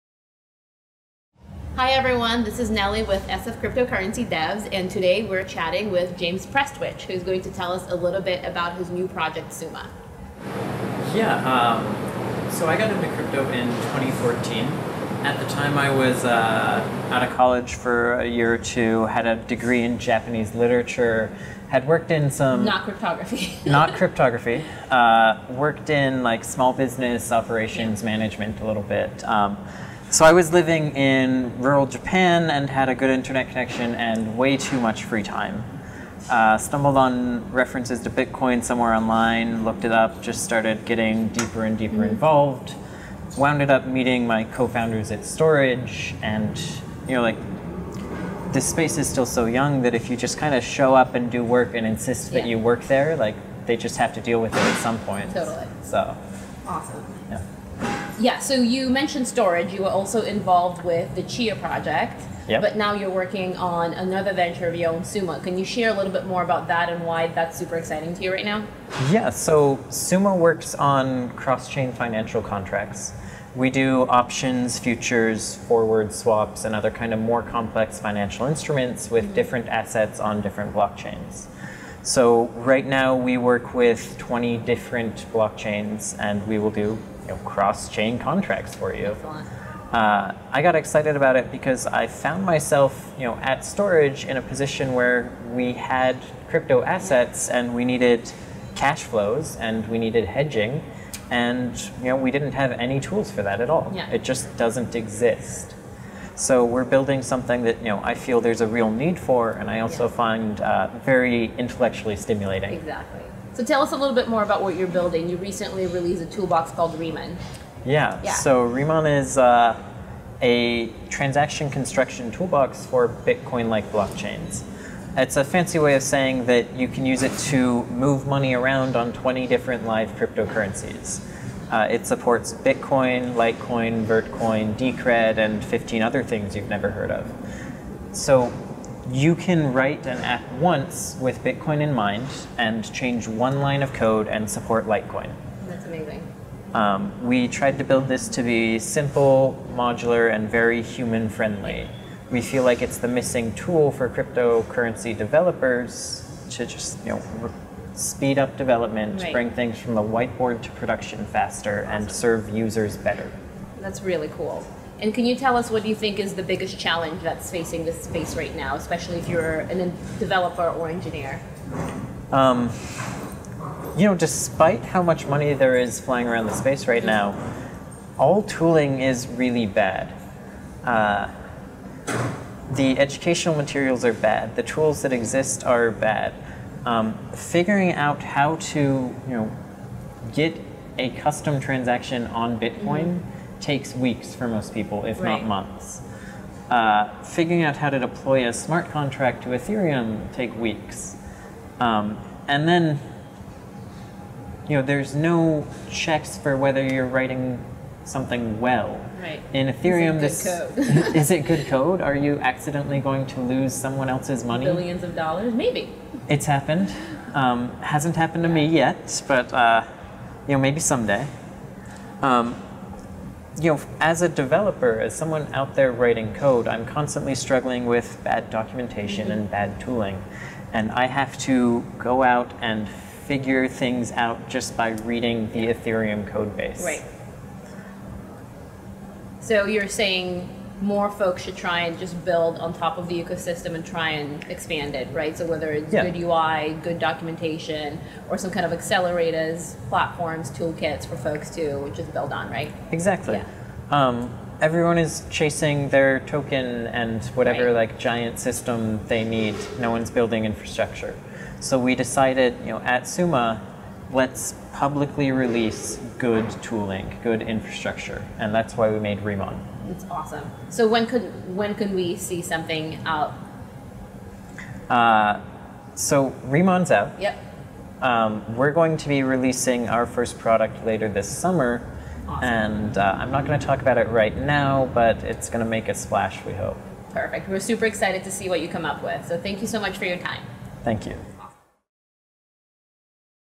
SFCDEVs Interviews: Nelly Mensah & James Prestwich's Summa: Atomic, Cross-chain Contracts
SFCDEVs Interviews: Nelly Mensah & James Prestwich's Summa: Atomic, Cross-chain Contracts and James'  history in the cryptocurrency space.